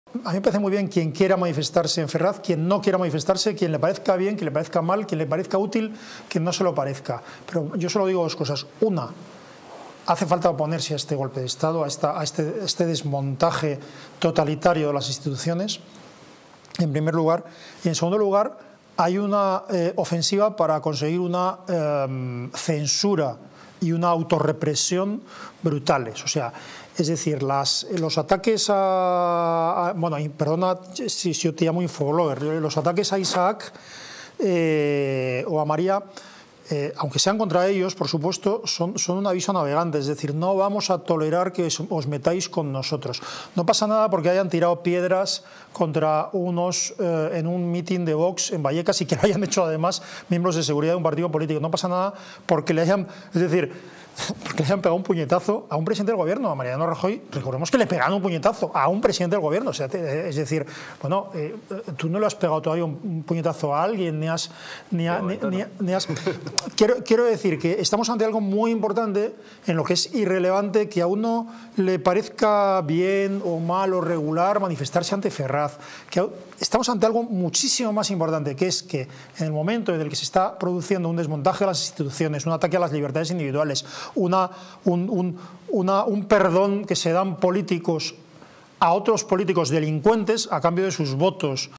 ASIS TIMERMAS: Hace falta oponerse a este golpe de estado
Hoy en el programa de Con España a Vueltas hablaremos de la piñata de Sánchez, reprobación Ortega Smith e ilegalización partidos independentistas con Asís Timermans y Isaac Parejo. En nuestra entrevista contaremos con la vicepresidenta de ACOM, Rosa Reigía para hablarnos de la guerra Israel-Palestina. Y también tendremos otra entrevista con el escritor Manuel R. Lavado para hablarnos de su libro “Quince Halcones”.